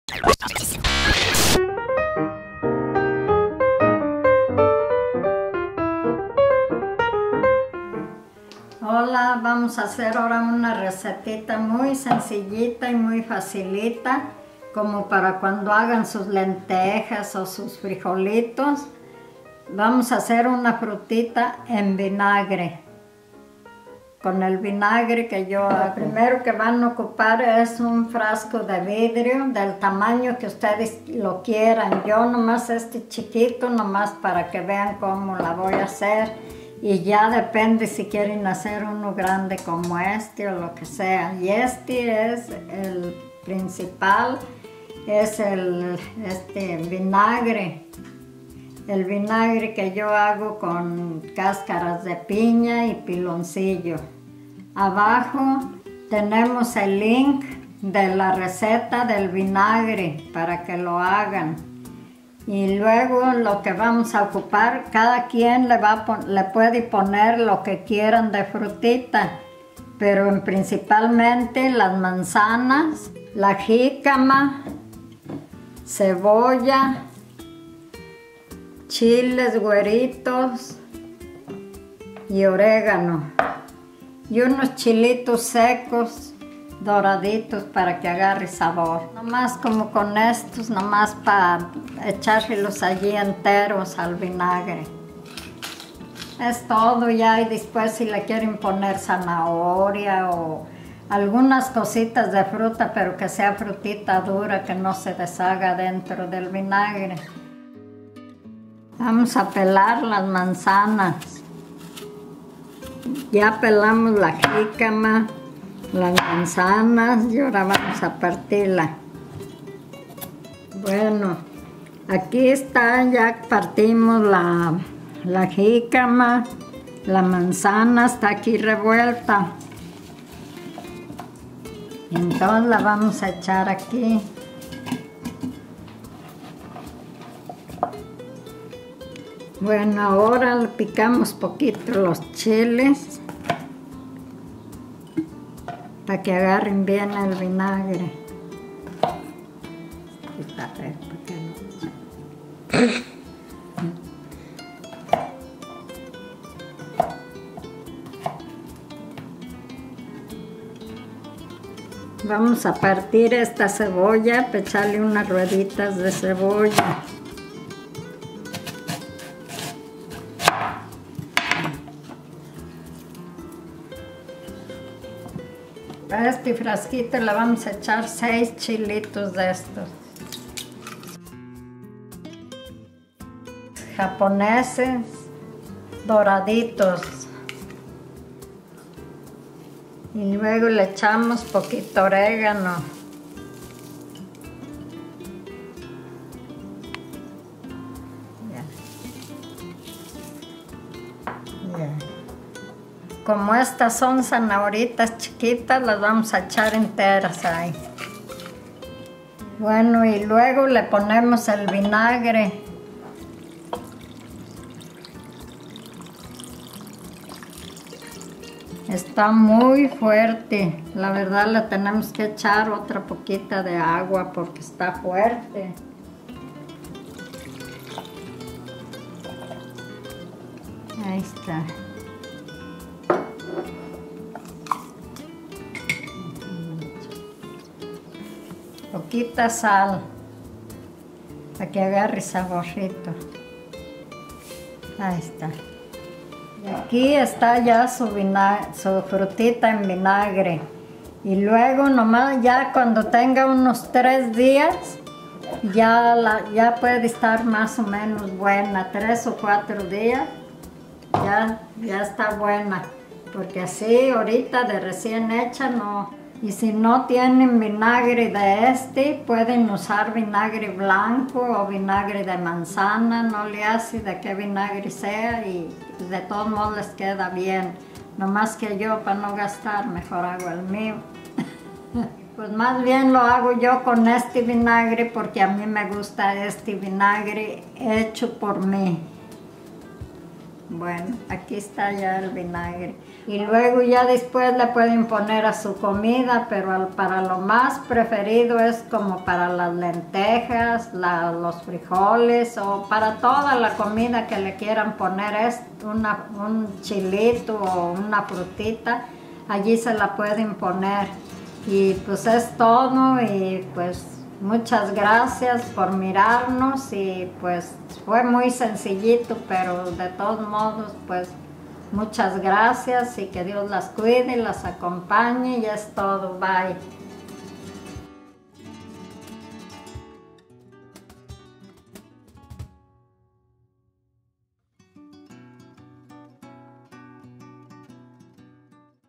Fruta En Vinagre (How To)
Abuela shows us how to make Fruit in Vinegar.